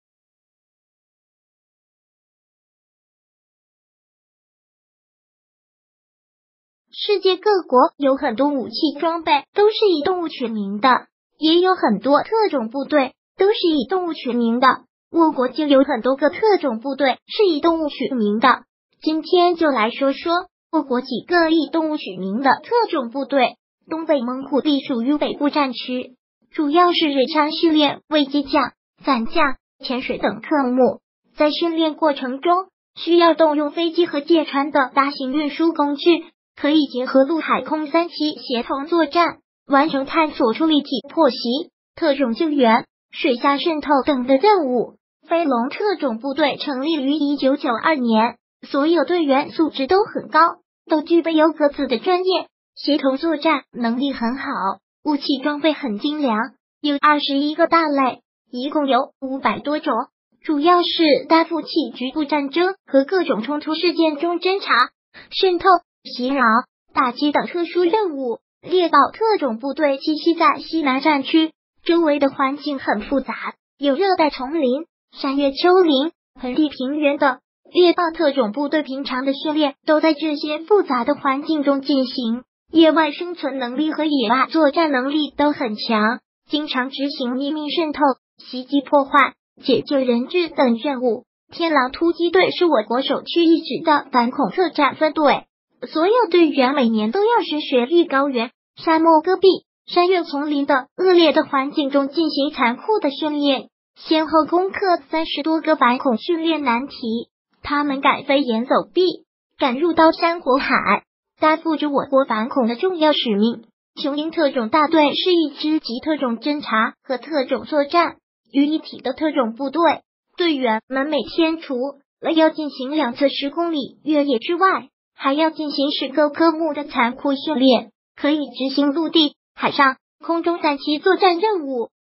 我国以动物取名的特种部队，你知道多少个呢？
世界各国有很多武器装备都是以动物取名的，也有很多特种部队都是以动物取名的，我国就有很多个特种部队是以动物取名的，今天就来说说我国几个以动物取名的特种部队。东北猛虎隶属于北部战区，主要是日常训练为机降、伞降、潜水等课目，在训练过程中，需要动用飞机和舰船等大型运输工具。可以结合陆海空三栖协同作战，完成探索出立体破袭，特种救援，水下渗透等的任务。飞龙特种部队成立于1992年，所有队员素质都很高，都具备有各自的专业，协同作战能力很好。武器装备很精良，有21个大类，一共有500多种。主要是担负起局部战争和各种冲突事件中侦察、渗透、袭扰、打击等特殊任务。猎豹特种部队栖息在西南战区，周围的环境很复杂，有热带丛林、山岳丘陵、盆地平原等，猎豹特种部队平常的训练都在这些复杂的环境中进行，野外生存能力和野外作战能力都很强，经常执行秘密渗透、袭击破坏、解救人质等任务。天狼突击队是我国首屈一指的反恐特战分队，所有队员每年都要去雪域高原、沙漠戈壁、山岳丛林等恶劣的环境中进行残酷的训练，先后攻克30多个反恐训练难题。他们敢飞檐走壁，敢入刀山火海，担负着我国反恐的重要使命。雄鹰特种大队是一支集特种侦察和特种作战于一体